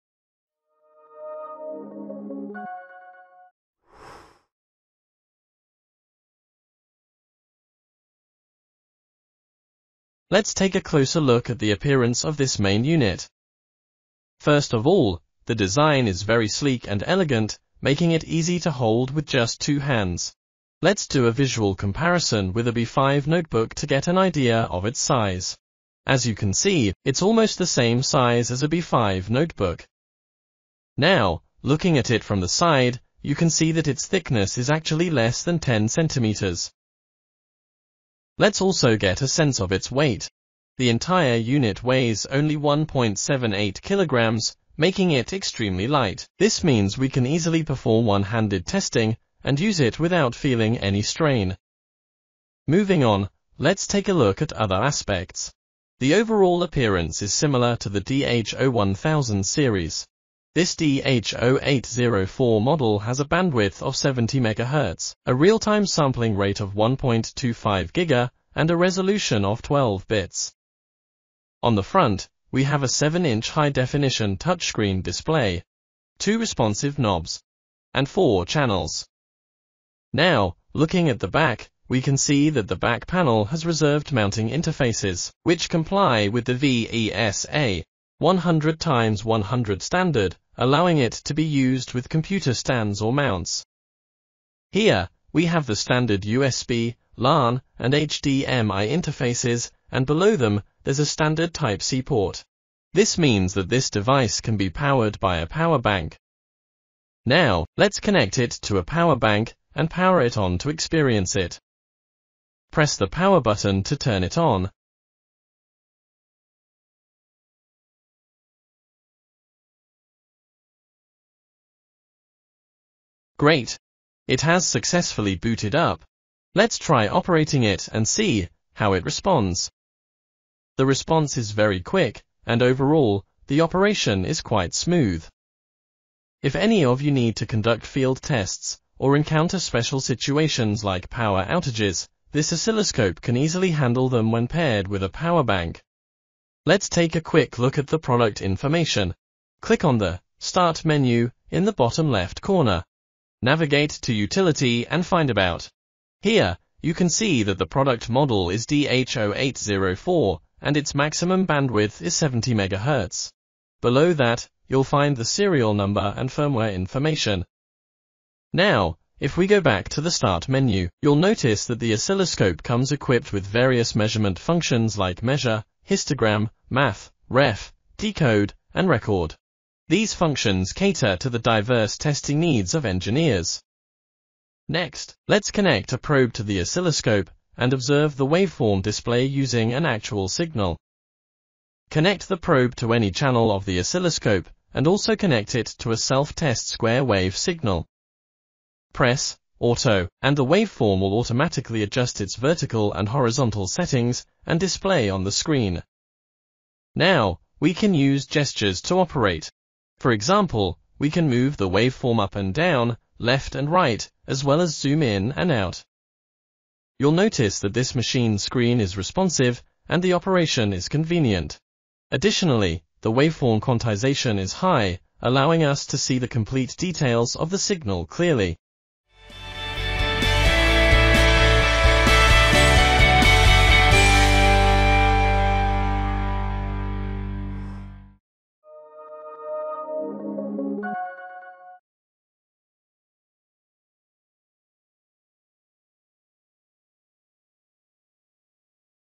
Rigol DHO800 12-Bit Oscilloscopes: Intro & Features from Saelig
The Rigol DHO800 Oscilloscope Series is a very compact high-performance 12-bit economical 70/100MHz digital oscilloscope range.  Not only are they very portable, with VESA mount capabilities to save bench space, but they offer the high-resolution needed to see fine signal detail. With a capture rate up to 1,000,000 wfms/s (in UltraAcquire Mode), and 25/50Mpts memory depth, the ultra-low noise floor of these scopes allows the detection of even small signal details.  Affordably priced, these scopes provide auto serial and parallel bus analysis, Bode plot analysis, and many other functions needed for today’s test demands in R&D, education, and scientific research.